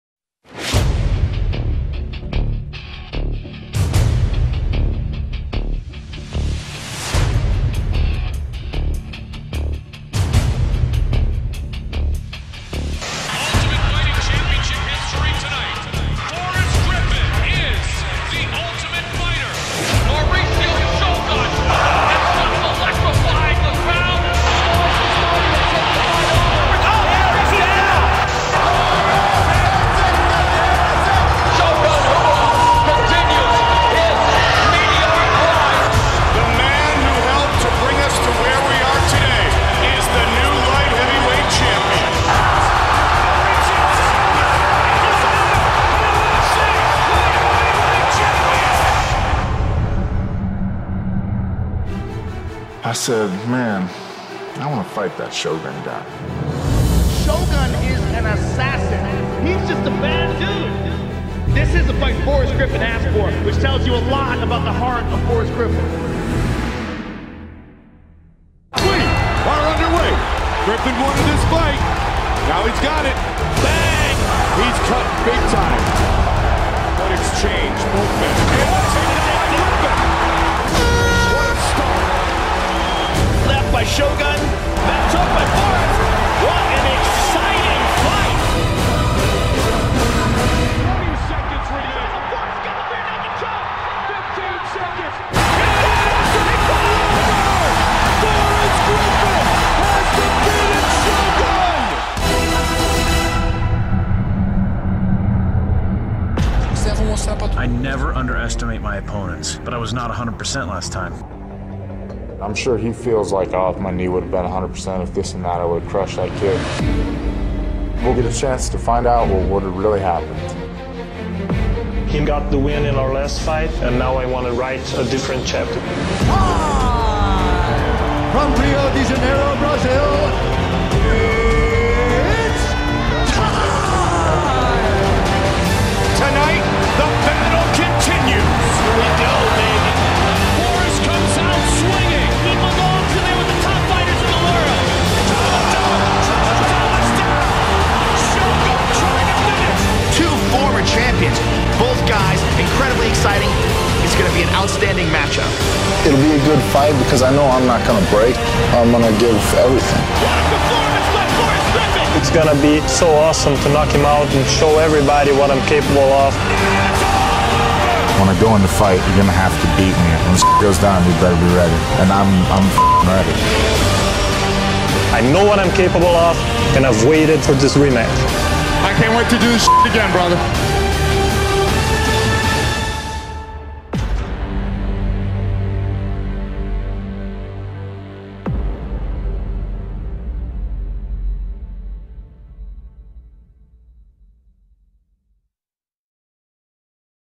Griffin vs Shogun 2 Trailer
Promo for the rematch between Griffin and Rua, who fight in the co-main event of UFC 134.

(thanks to @SCavax88 for finding that one)